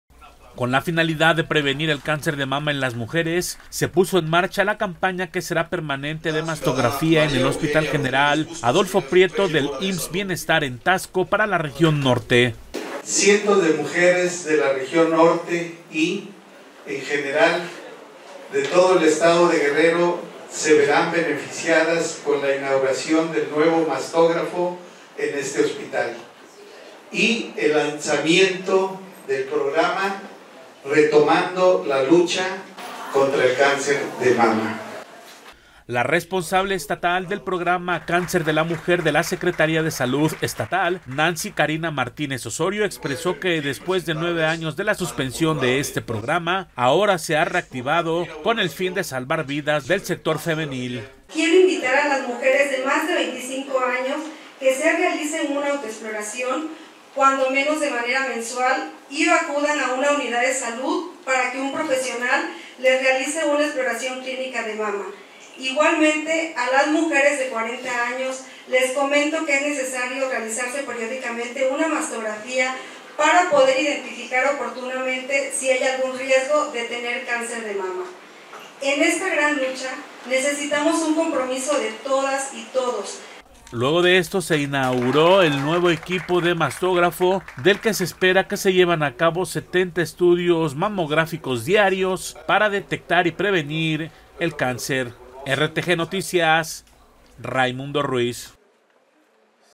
RTGnoticias | Arranca campaña permanente de mastografía en Taxco
Con la finalidad de prevenir el cáncer de mama en las mujeres, se puso en marcha la campaña que será permanente de mastografía en el hospital general Adolfo Prieto del IMSS-Bienestar en Taxco para la región Norte.